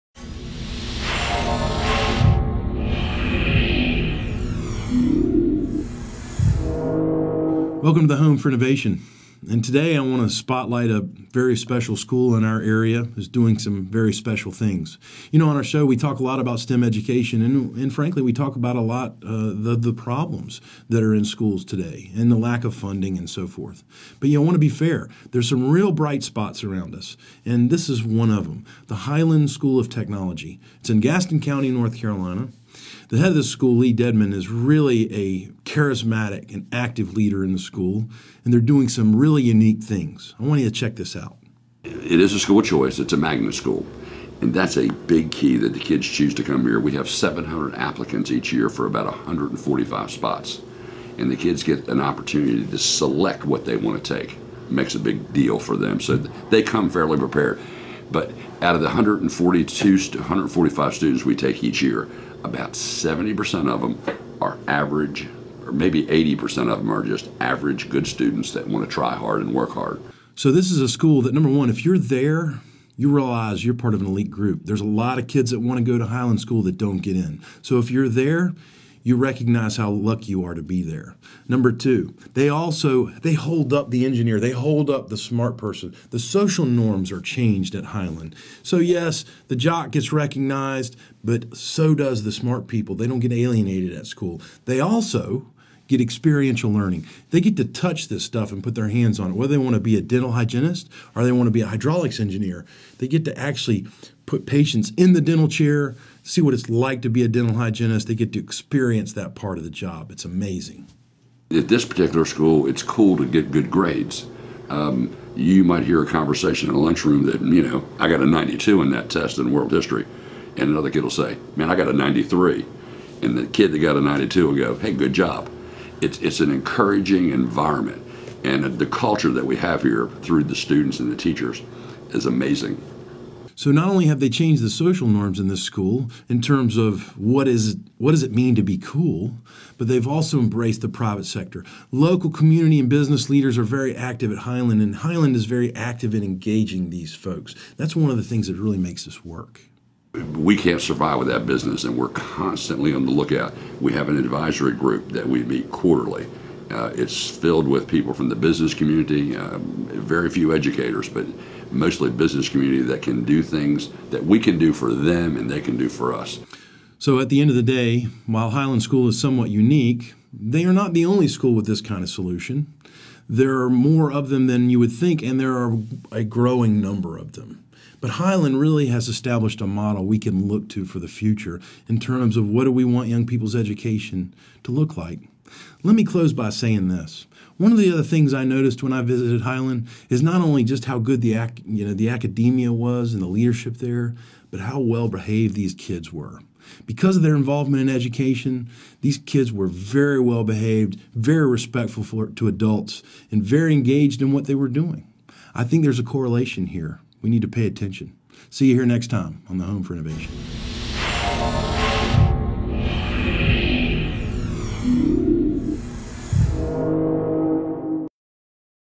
Education Spotlight: Highland School of Technology
Clifton spotlights the Highland School of Technology in Gaston County, NC, a magnet school that has a formula for success that should be modelled by schools throughout the U.S.